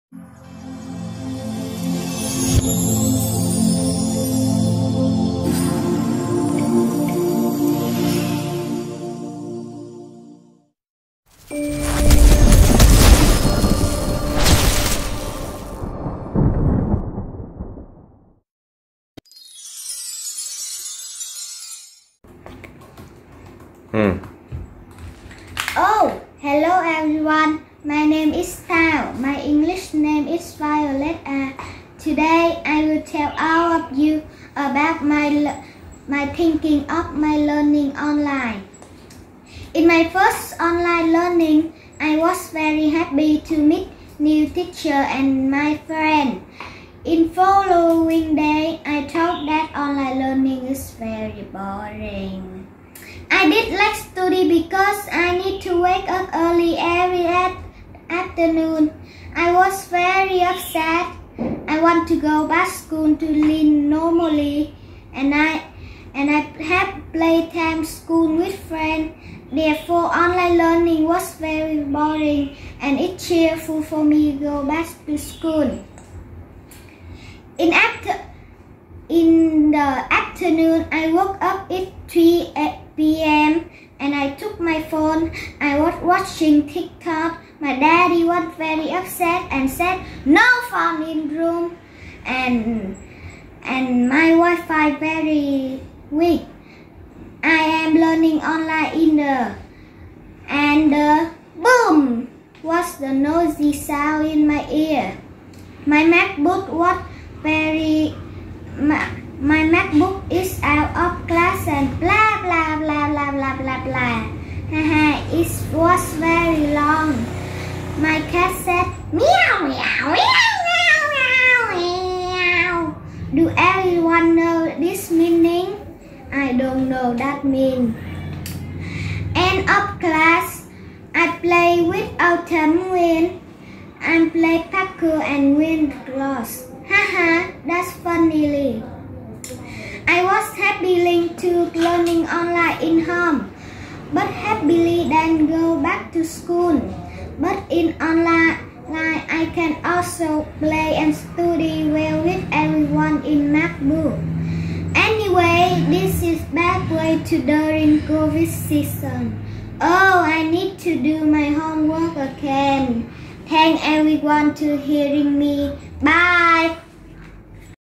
#21 Hùng biện Tiếng Anh 2021-2022_Nguyễn Hoàng Xuân Thảo_Năm 2
Đến với cuộc thi hùng biện Tiếng Anh 2021 kỳ này, các em sẽ thảo luận về câu hỏi:
"What do you think about Online learning?"
"Suggest some ways to have an effective lesson."
Phần dự thi của học sinh: Nguyễn Hoàng Xuân Thảo
Lớp: Năm 2